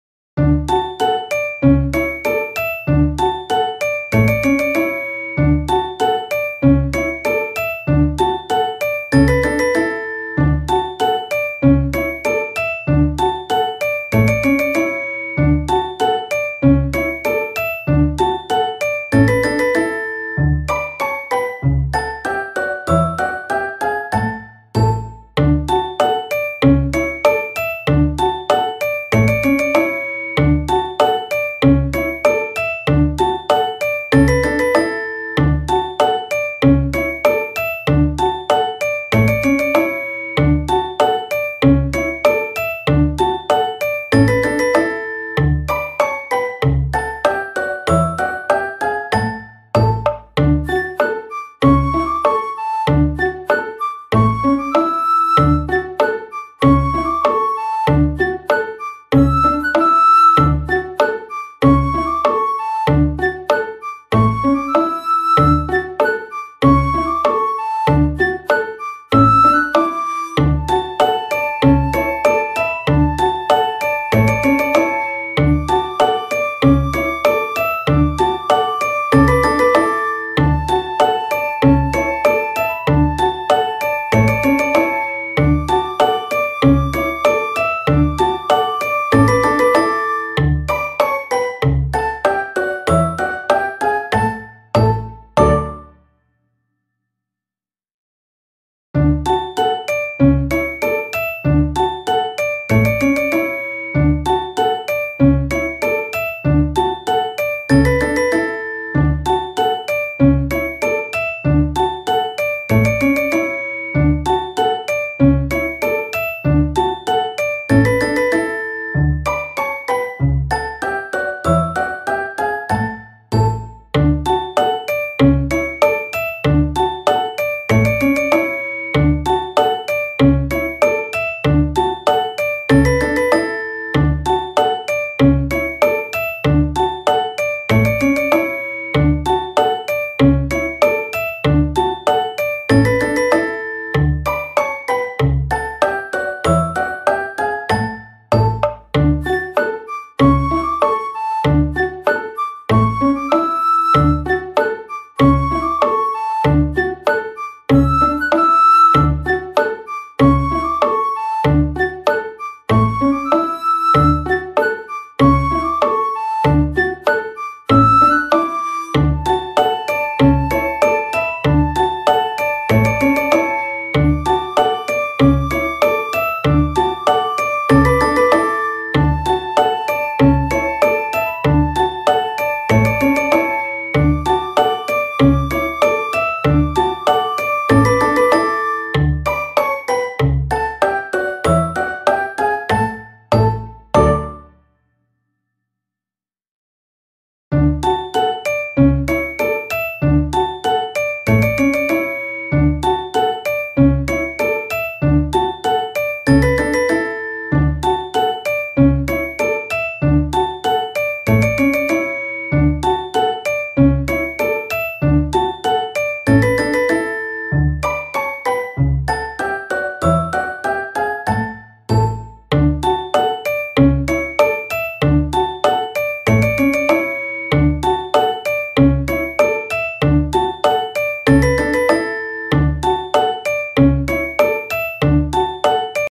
的野美青『浮気現場💔💦』
櫻坂46 3期生 的野美青があるメンバーから好意を寄せられて期待に応える。しかし、櫻坂のイケメンに浮気は付き物？イケメンな的野君は別のメンバーにも手を出してしまうのでした...💦可愛いので是非ご覧ください！